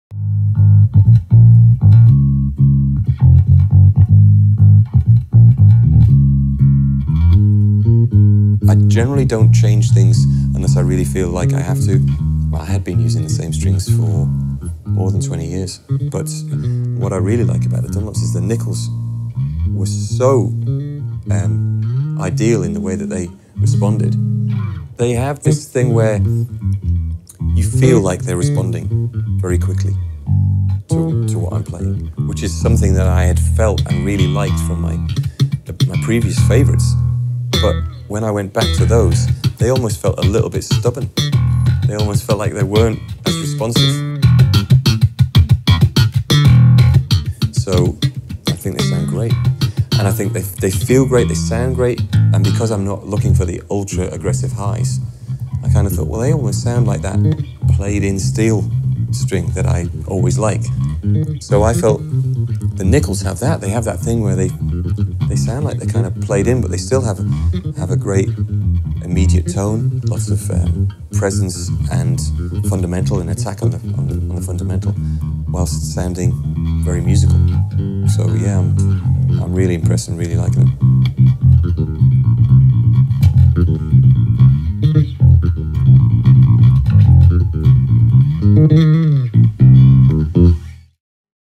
Dunlop Super Bright Bass Strings: Paul Turner
Renowned UK bassist Paul Turner (Jamiroquai, Dark Sinatras, Annie Lennox, Lamont Dozier) has spent his whole life dedicated to honing his craft and his tone. So when it comes to introducing changes to his gear, he takes things very seriously and doesn't make decisions on a whim.

For a player who has used the same bass for 15 years, the same amps for 10, and, up to now, the same brand of strings for 20, it was a great honor for Dunlop that our new Super Bright Nickel Bass Strings stood up under Paul's careful analysis. He loves what they bring to his playing, giving him the response, feel, and tone that he sees as an upgrade from his previous favorites.

SPECIAL THANKS:
Paul Turner
The Hospital Club, London